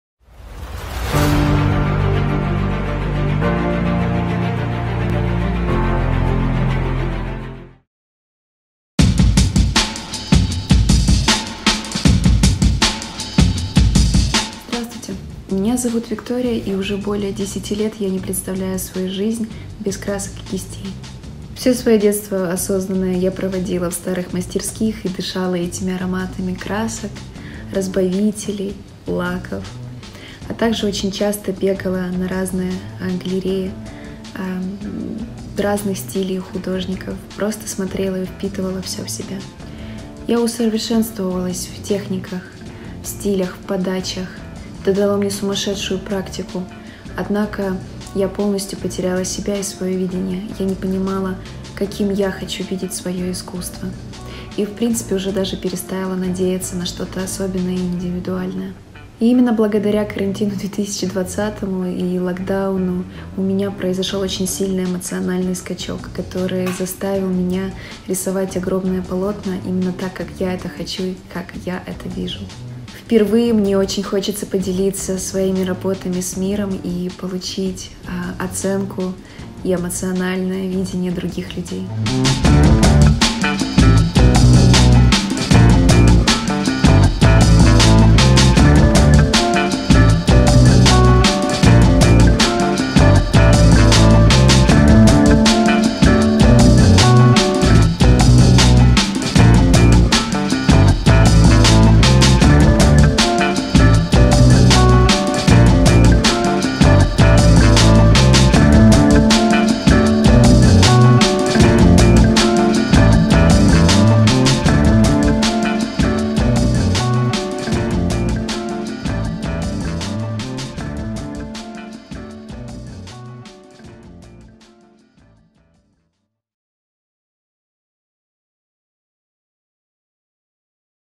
Golden Time Distant Festival | 19 Season | Viktoriia Marushchak | GT19-7727-2462
Имя участника или коллектив: Viktoriia Marushchak

Страна: Ukraine

Название конкурсной работы: The circle

Размеры работы: 150x160x20

Материал исполнения: Acrylics and canvas

История создания работы: I have been improving my fine art skills for the last ten years by studying different technics, working with many artists, had experience learning abroad.

Alas, I could not feel my voice in the global world of Art and almost lose the hope to get one. However, the situation with coronavirus gave me a developing point, which helped to understand what do I want to express with my artwork.

Номинация: Arts and crafts

Возрастная категория: over 20 years old

19 Season

Участие в других конкурсах и фестивалях / творческие достижения: Five years have been studying in the UK.
I have completed the A-level programme in 2016. The main specification of that course was Art and Design Technology.

During that period, I have participated in different competition as a graphic designer and furniture manufacturer.

I have graduated as an architect student at Cardiff University in 2020.
Working as a commercial photographer for the last two years.

The fine art was always the best way for me to express myself.
Participation in this competition is my debut in showing my artwork to the public.